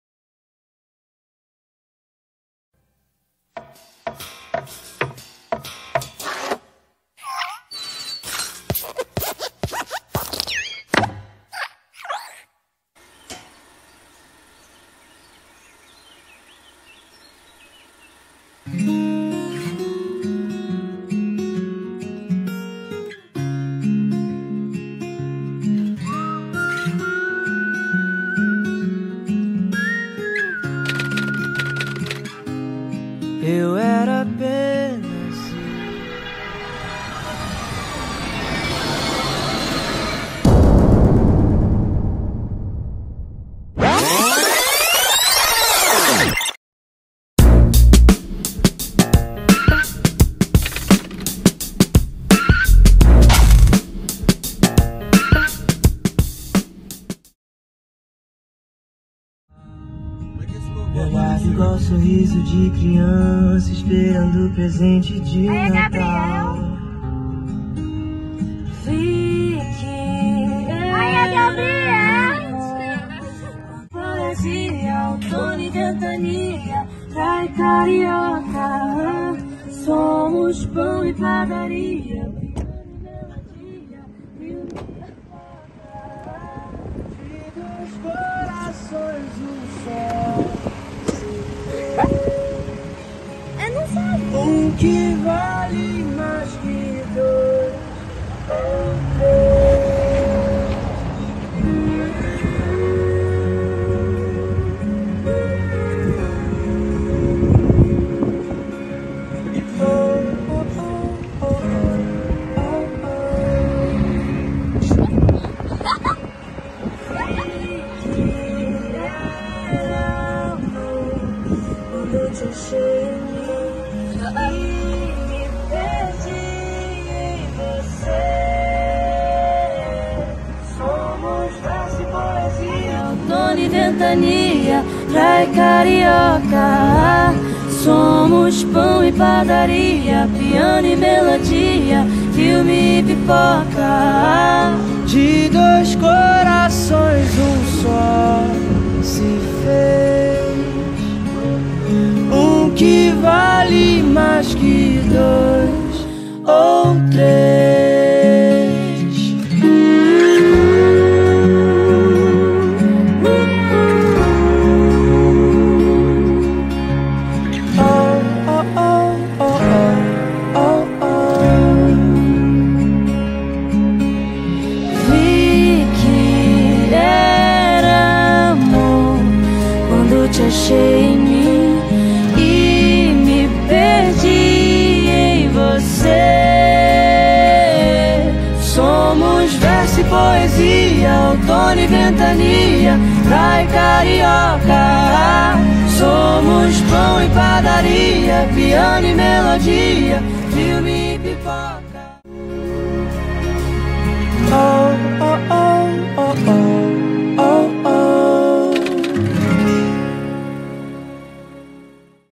PEDIDO DE NAMORO
Este video mostra a história de um pedido de namoro. Muito emocionante, mostra o momento em que Gabriel Sodré pede Hannah Carvalho em namoro. no dia 18/12/2023. exatamente as 18:05 até o por do sol.